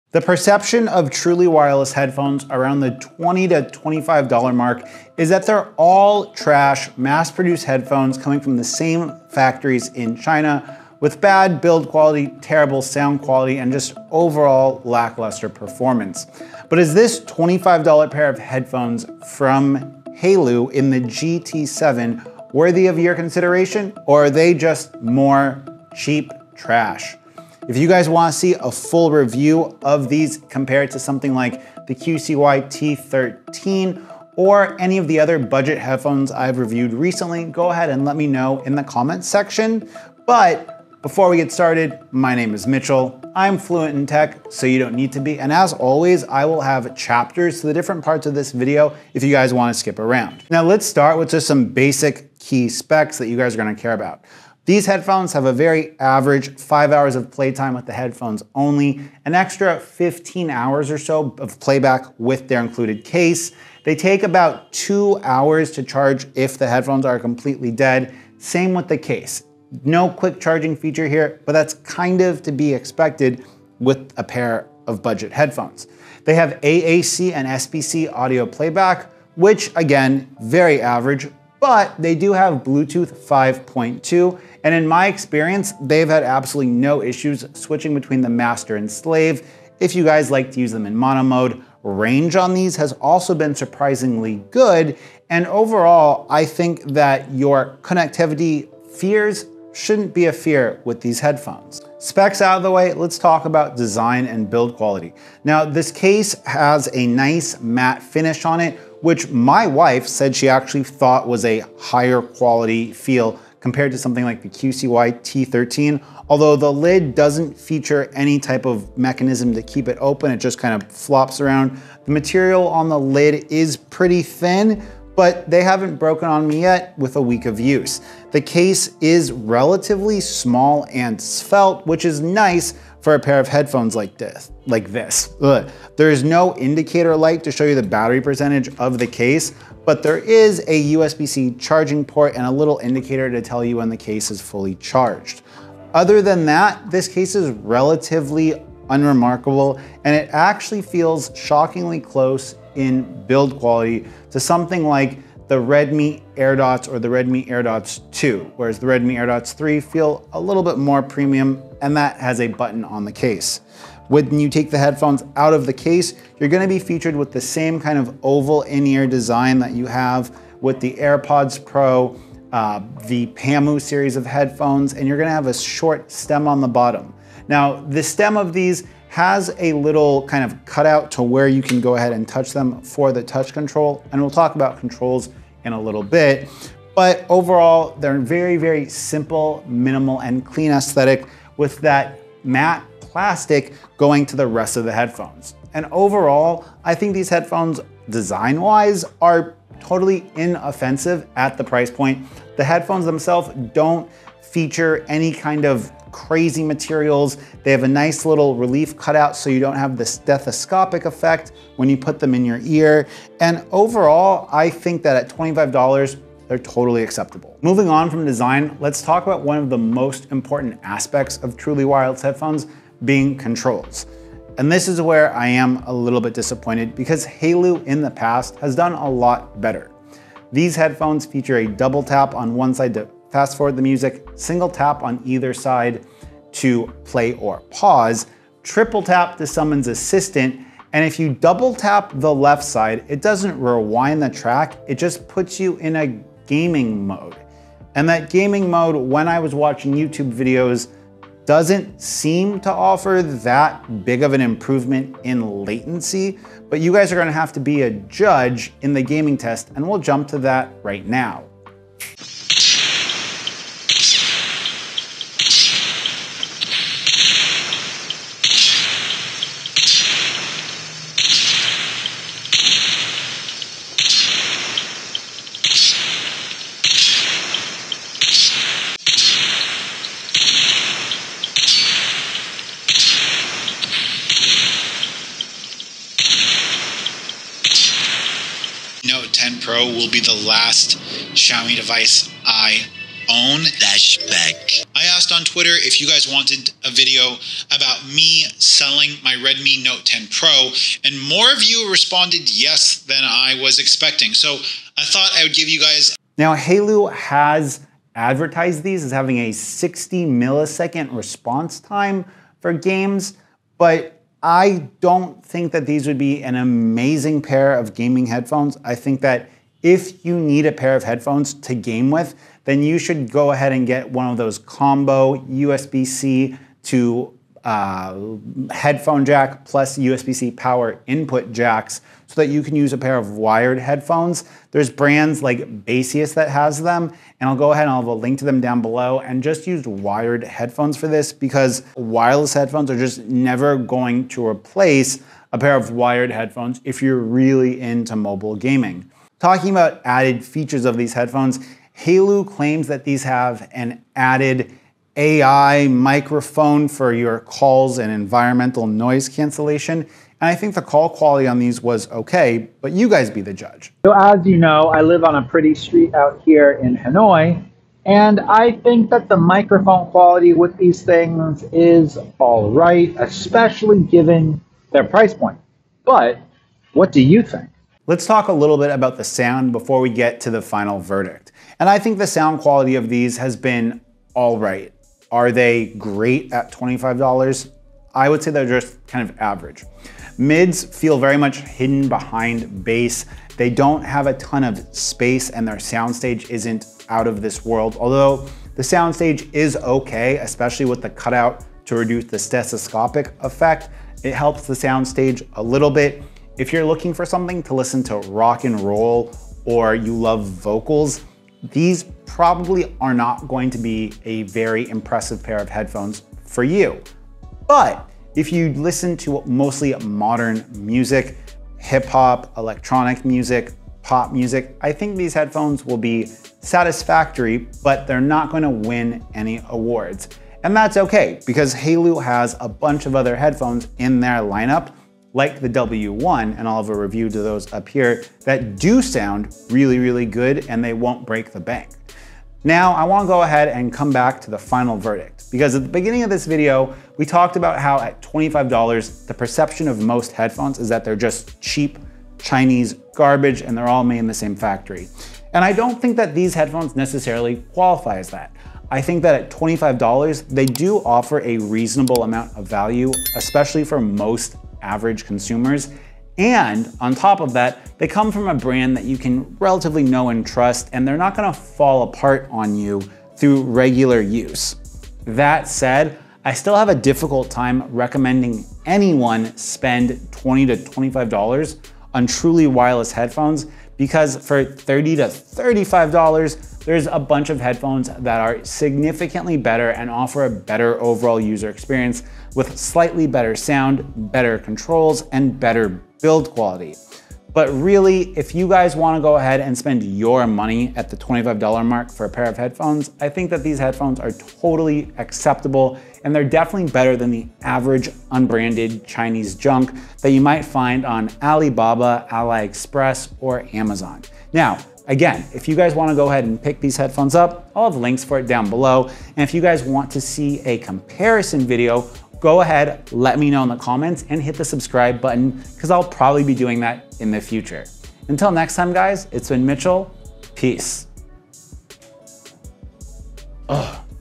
Haylou GT7 Review, Trash Or Treasure?
I have reviewed a lot of cheap wireless headphones and quite a few pairs of headphones from Haylou. In today's review of the Haylou GT7 we find out if the Haylou GT7 are worth your $25 or if the Haylou GT7 are just another pair of cheap Bluetooth headphones. If you are looking for a comparison between cheap wireless headphones such as the Haylou GT7 and this review of the Haylou GT7 wasn't enough comment on how I could review the Haylou GT7 better.

Buy The Haylou GT7 From The Official Haylou Store On Ali Express

0:00-0:40 Haylou GT7 introduction
0:41-1:46 Haylou GT7 Specs
1:47-3:41 Haylou GT7 build quality and design
3:42-4:20 Haylou GT7 Controls
4:21-5:50 Haylou GT7 Gaming and Latency
5:51-6:21 Haylou GT7 Mic Test
6:22-7:27 Haylou GT7 Sound Quality
7:28-8:54 Haylou GT7 Final verdict